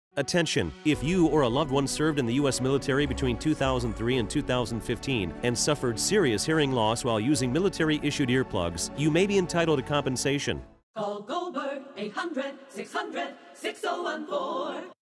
Military Hearing Loss Lawsuit Attorney | Call Goldberg
Have you been diagnosed with tinnitus or hearing loss?

If you wore Combat Arms™ Earplugs (CAEv2) between 2003 and 2015 during foreign conflicts, you may be entitled to significant compensation.

In July 2018, there was a whistleblower complaint that settled. 3M admitted to a design defect in their product and paid $9.1 million to resolve the issue. The defective Combat Arms Earplugs ™ (CAEv2) were sold to the military for more than a decade. Combat Arms Earplugs™ have a dangerous design that can cause them to loosen while during use. This defect can cause hearing damage or tinnitus, which represents the largest ongoing medical cost to the military.

Do you suffer from severe ringing in your ears? Did you serve during any of the following conflicts?

A defense contractor knowingly put service members at risk for hearing loss. Veterans nationwide have suffered partial or total hearing loss because of defective earplugs. The dual-ended 3M Combat Arms™ Earplugs were standard military issue for soldiers from 2003 to 2015.

War of Afghanistan
The Iraq WarWar in North-West Pakistan (part of War on Terror)
War in Somalia
Operation Ocean Shield in the Indian Ocean
American-led intervention in Libya (2011- part of Libyan Crisis)
American-led intervention in Iraq (2014 – 2017)
American-led Intervention in Syria (2014 to present)
Yemeni Civil War (2015 to present)
American Intervention in Libya (2015 to present)

At Goldberg Attorneys, we truly understand the impact of defective products. Our law firm is here to help you.